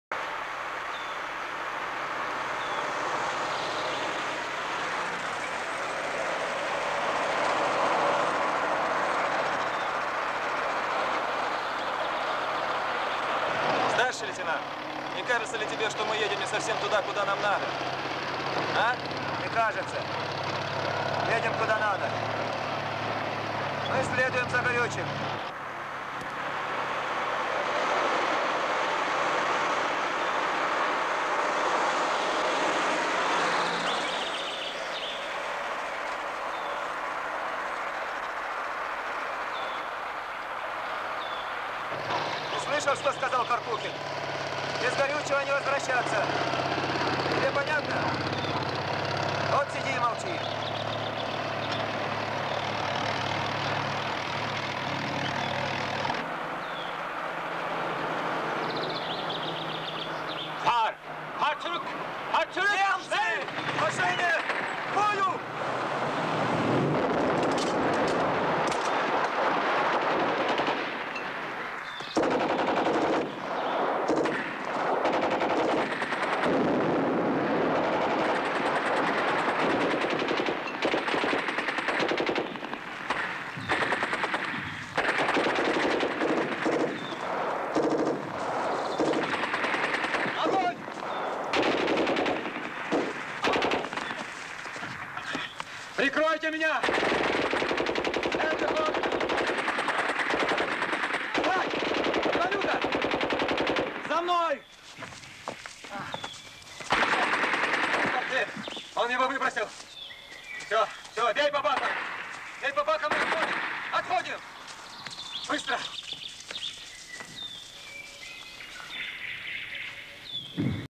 action on the road German officer captured; his guard killed
This could mean war!